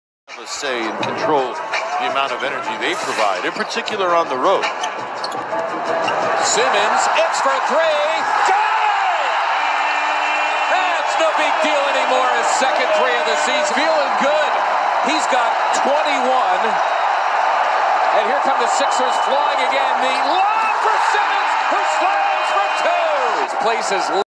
Ben Simmons' SECOND 3 POINTER Of The Season Followed With an Ally Oop!
This is ben Simmons' second three of the season